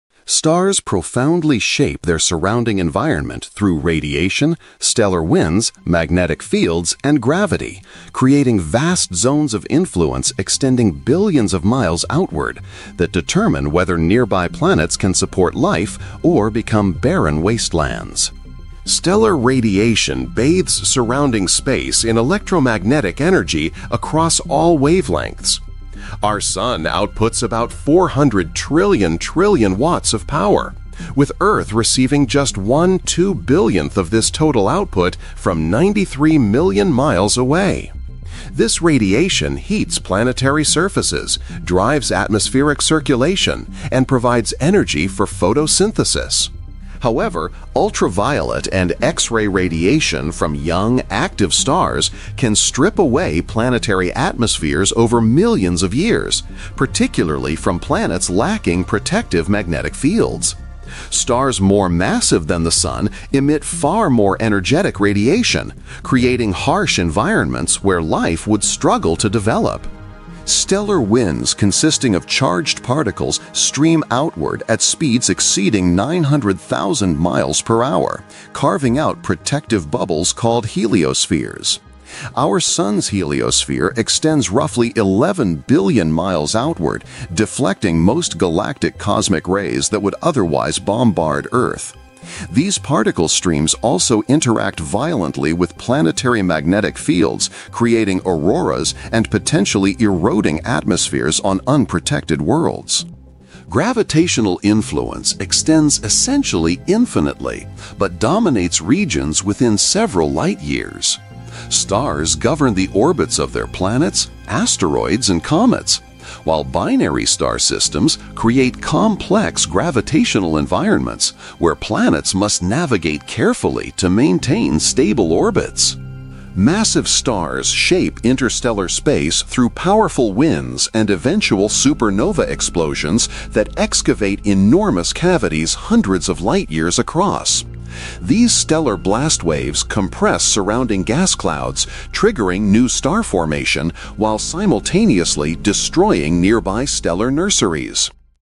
How Do Stars Affect Nearby Space?
Stars profoundly shape their surrounding environment through radiation, stellar winds, magnetic fields, and gravity, creating vast zones of influence extending billions of miles outward that determine whether nearby planets can support life or become barren wastelands.

Try Amazon Prime for fast shipping, streaming, and exclusive deals.
(paid link)